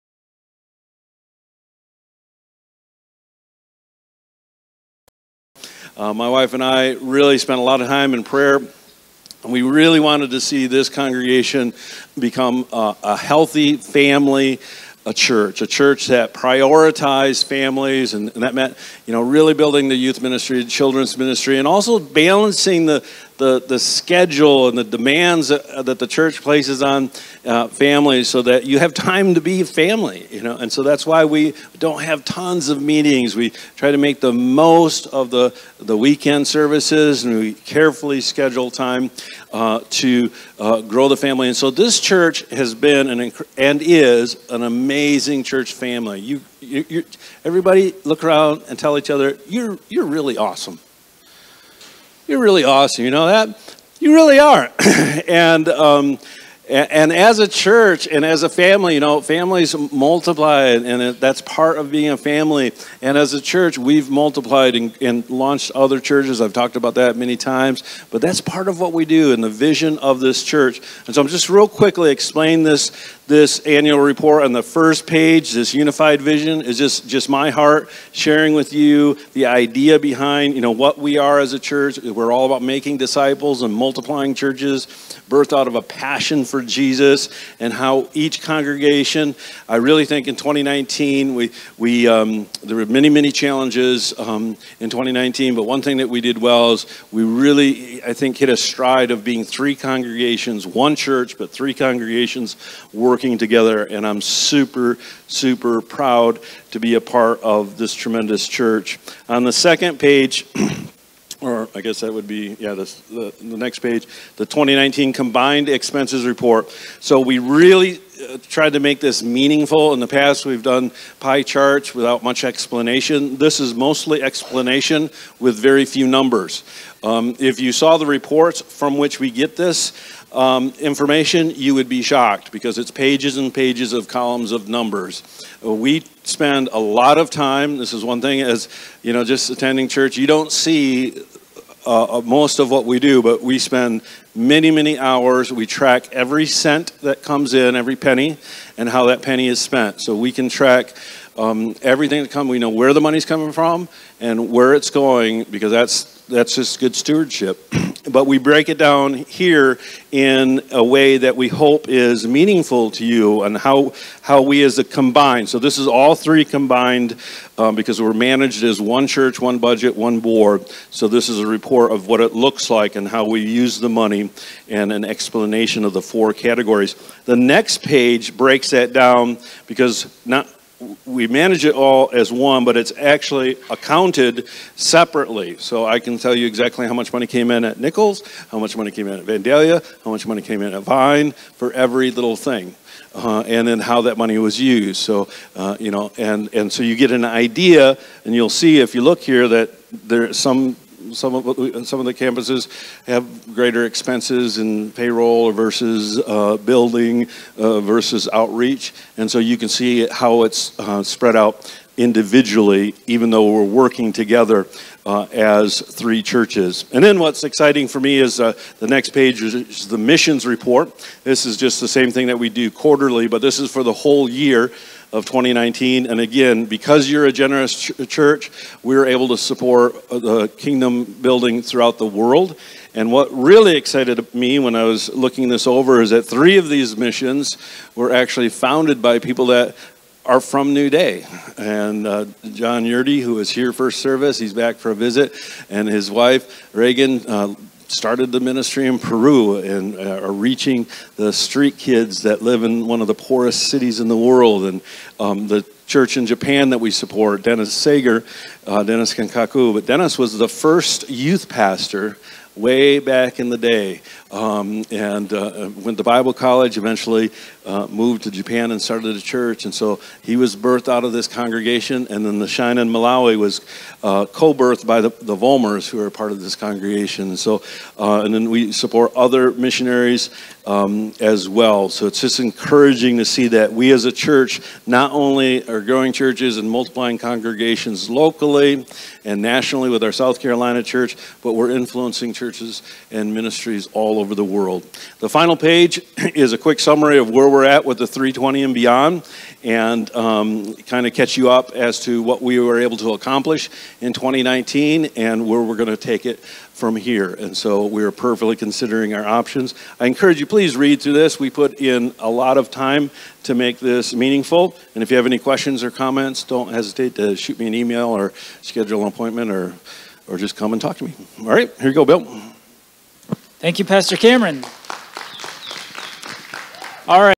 2019 Year End Financial Report | Pastor Cameron
Today, we released the 2019 year-end financial report.Below is a link to a pdf copy.  In it, you’ll find a personal letter from Pastor Cameron, as well as a breakdown of where your money went in 2019—at our church, domestically, and globally. Thank you so much for your financial generosity!